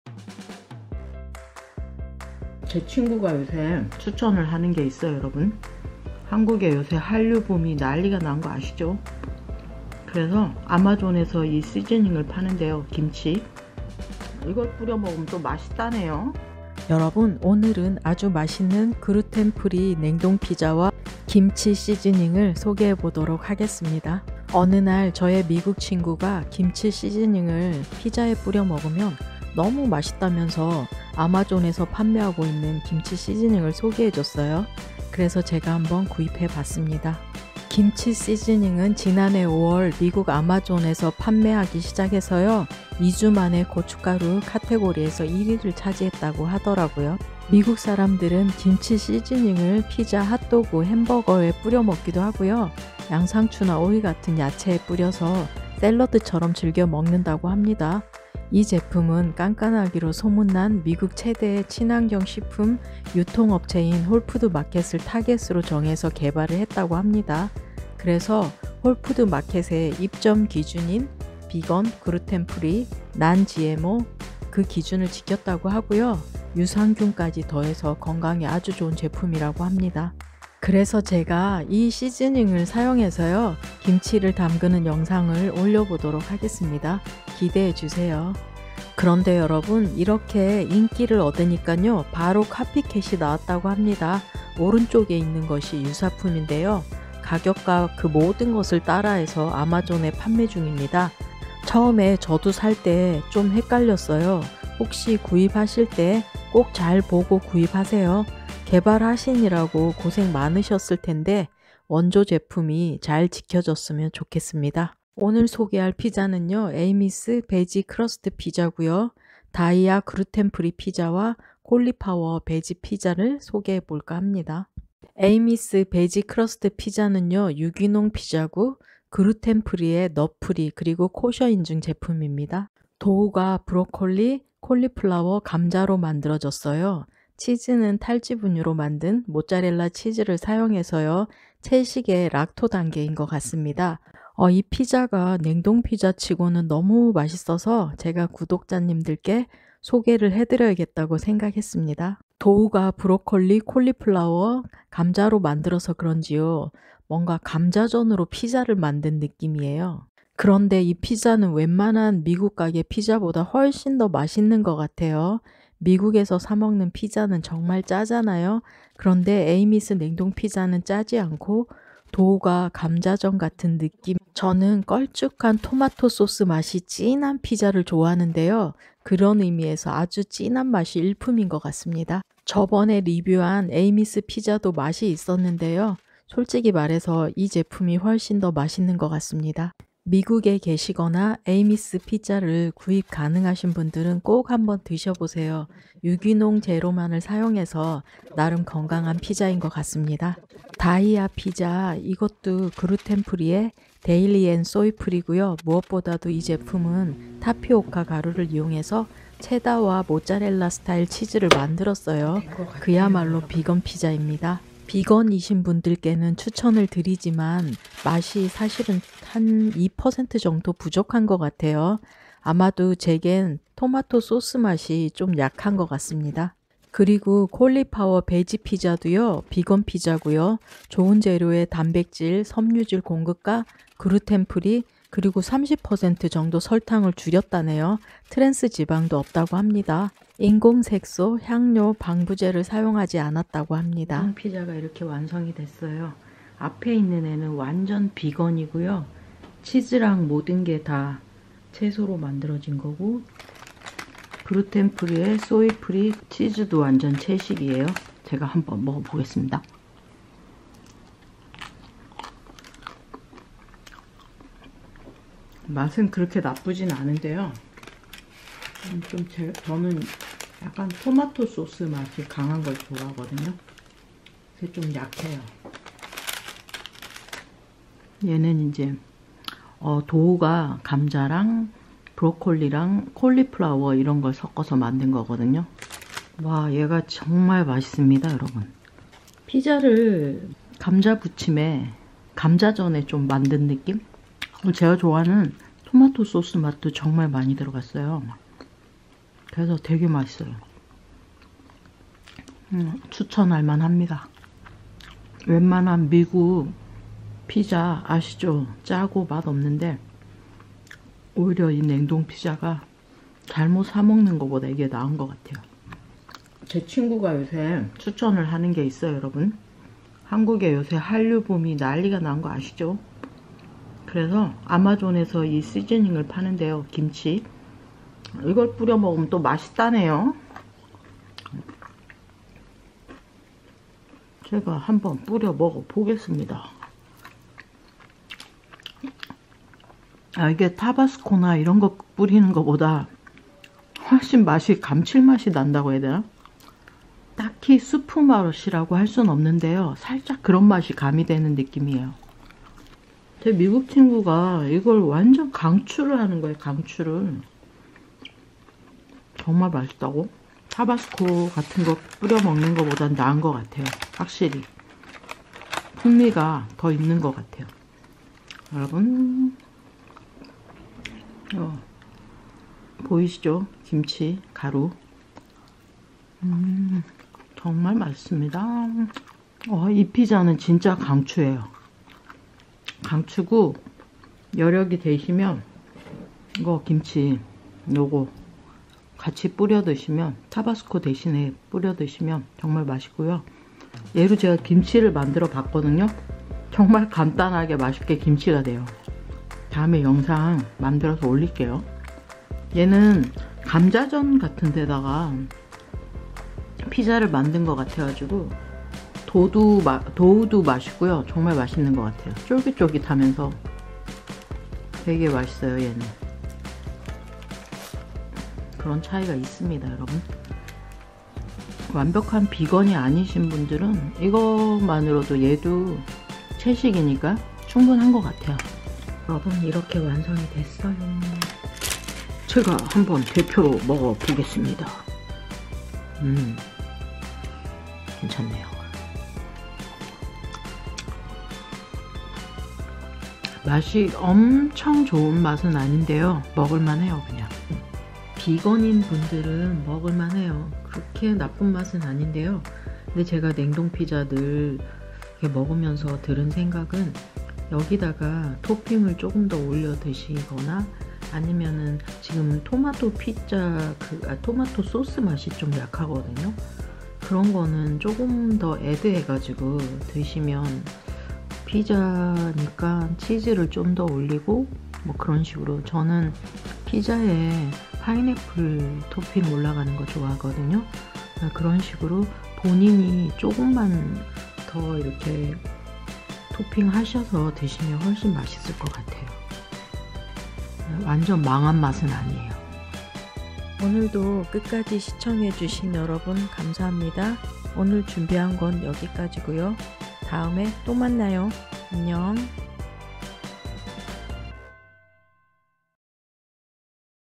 [드림웍스이야기]미국인들이 꽂힌 김치맛 K시즈닝/아마존판매1위 김치시즈닝/냉동베지피자/비건/글루텐프리Tree nut, Dairy &soy free/Dreamer오늘
19년차 드림웍스에 출근하고있는 Dreamer오늘입니다
30년후의 나를 위해 열심히 살아가는 흔적을 남기려합니다
30년후의 나에게 선물을 준비합니다
*너무 맛있는 에이미스 베지 크러스트(Amy's Veggie Crust) 피자와
요즘 미국인들이 꽂힌 아마존 판매1위 김치시즈닝(Kimchi Seasoning)을 소개합니다
이제 시즈닝도 한류입니다
타바스코 스리라차에서 김치시즈닝으로 갈아 탔답니다 .
피자는 홀푸드, 아마존, 트레이더조, 타겟, 월마트,vons, instacart에서
구입가능!